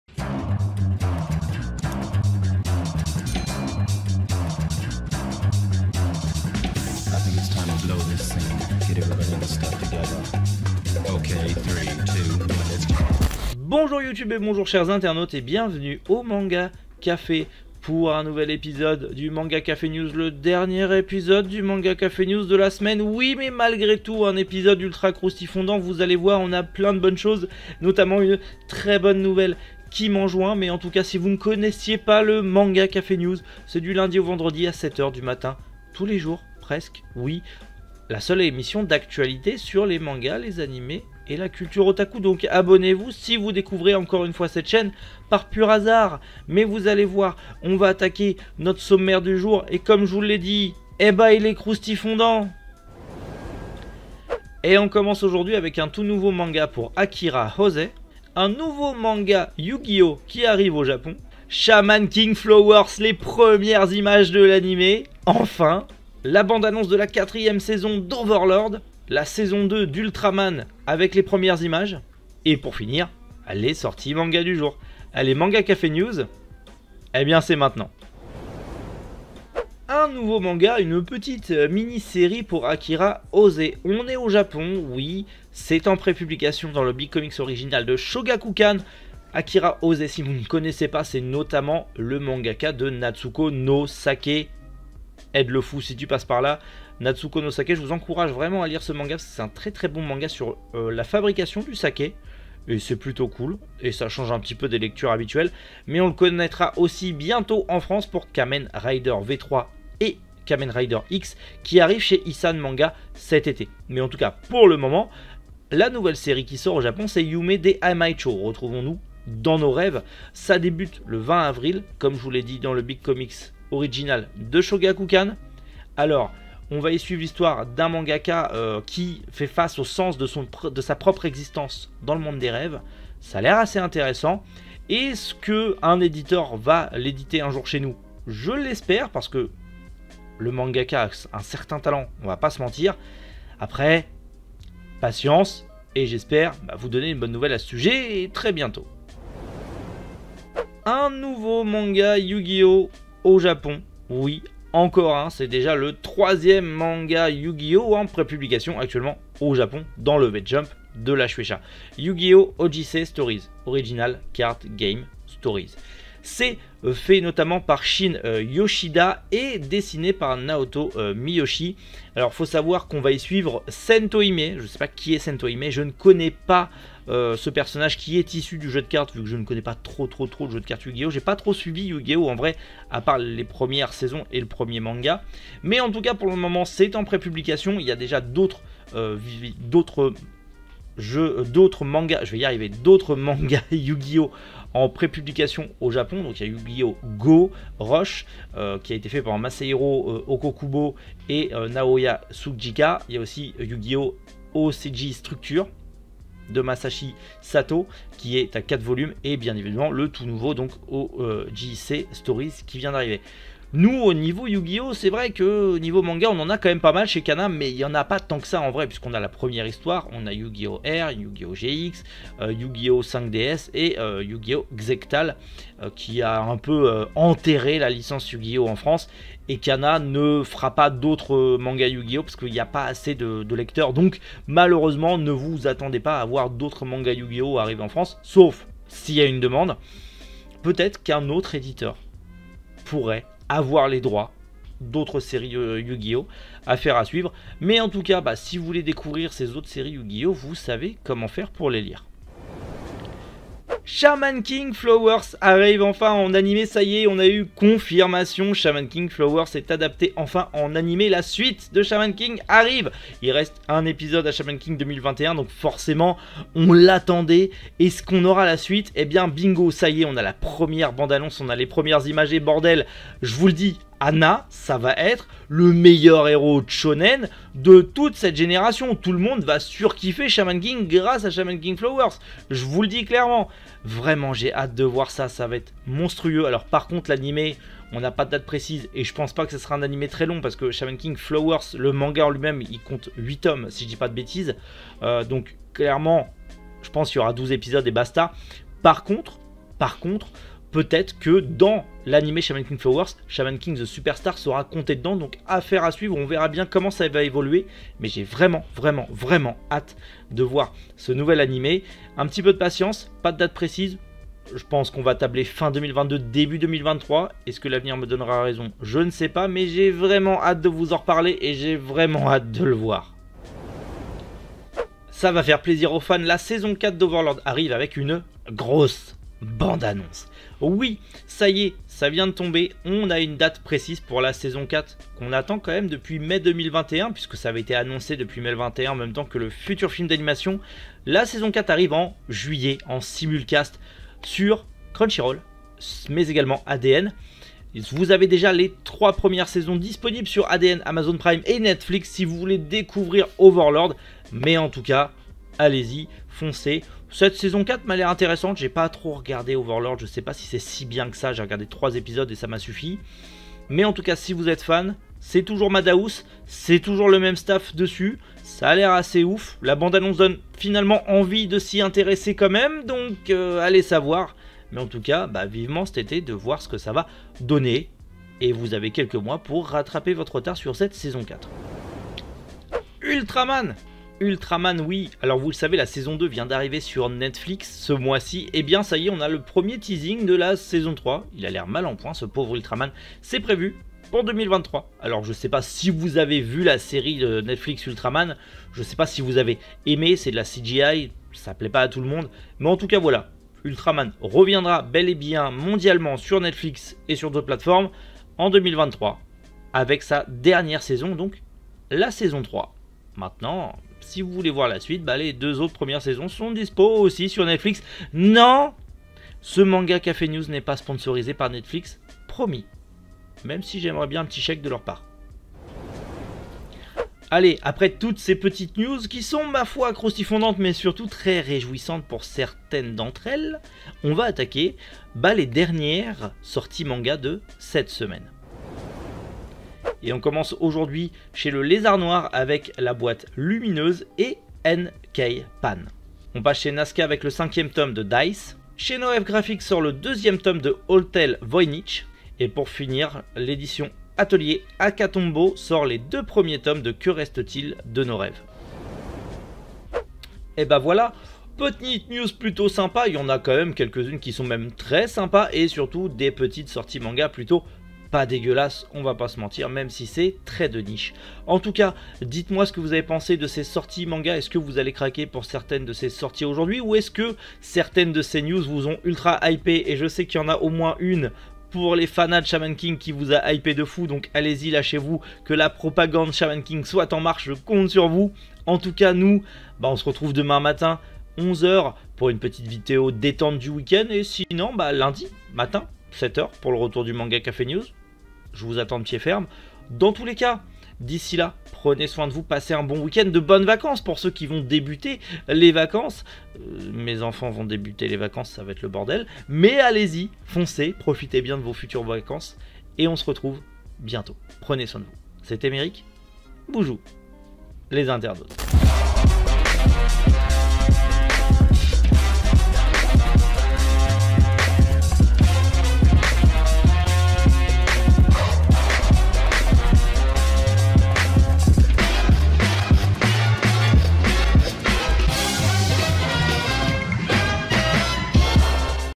Un nouveau manga Yu-Gi-Oh!⭐La saison 4 d'Overlord💀Shaman King Flowers adapté en animé👻
Bonjours Youtube et bonjours chers internautes, bienvenue au Manga café, aujourd'hui de nouvelle news toute fraiche !
Bon visionnage !
00:00 - intro
01:16 - Un nouveau pour Akira Oze
02:29 - Un nouveau manga Yu-Gi-Oh!
04:12 - Shaman King Flowers adapté en manga
05:23 - La saison 4 d'Overlord
06:22 - La saison finale d'Ultraman
07:06 - Les sorties manga du jour
07:39 - Conclusion
Venez voir ou revoir les lives sur la chaine secondaire :
Mon manga collec:
Nos partenaires :
Lien utile:
Bannière réalisé par Anthox Colaboy
Logo réalisé par Shifu
Transition réalisé par Wanted